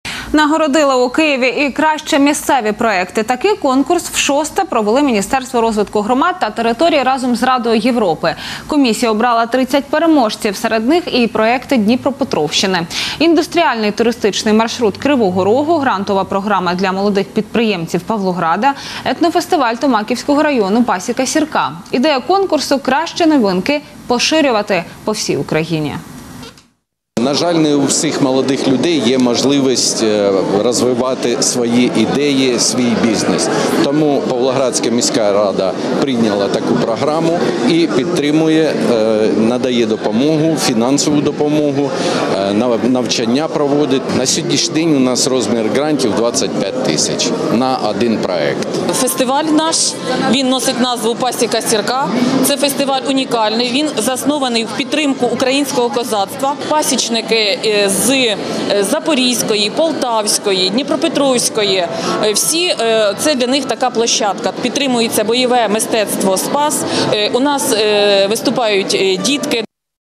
Проекты громад Днепропетровщины победили на конкурсе от Минрегиона
В Киеве наградили лучшие местные проекты. Такой конкурс в шестой раз провели Министерство развития громад и территорий совместно с Советом Европы.